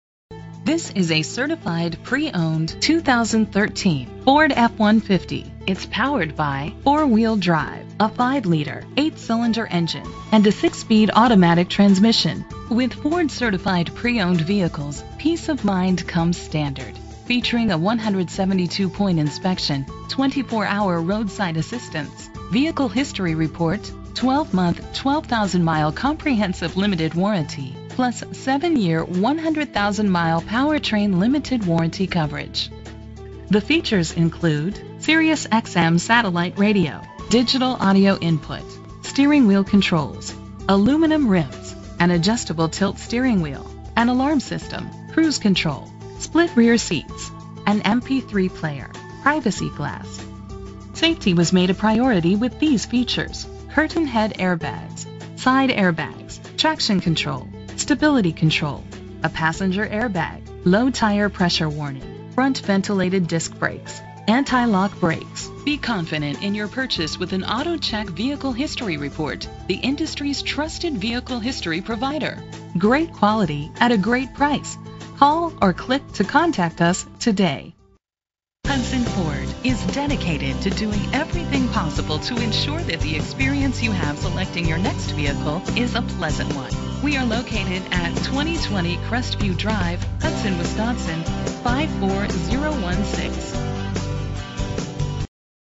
2013 Ford F-150 - Hudson WI
Phone:
Year: 2013
Make: Ford
Model: F-150
Trim: XLT
Engine: 5.0 liter 8 cylinder 32 valve
Transmission: 6-Speed Automatic
Color: Tuxedo Black Metallic
Mileage: 46613
Address:
2020 Crestview Drive